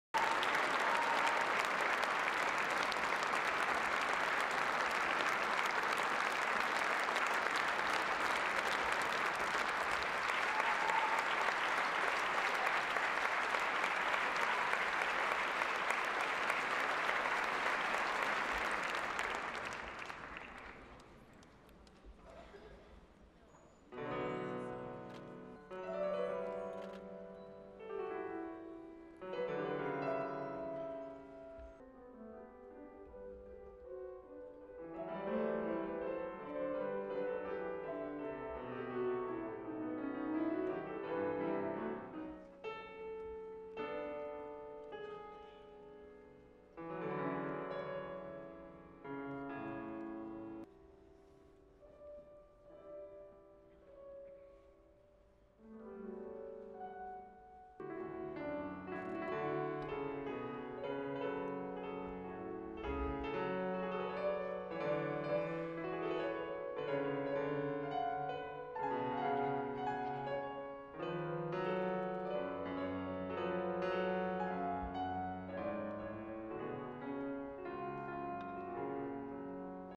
Armenian President attends concert of pianist Svetlana Navasardyan
Armenian President attends concert of pianist Svetlana Navasardyan/Հայաստանի նախագահը ներկա է գտնվել դաշնակահար Սվետլանա Նավասարդյանի համերգին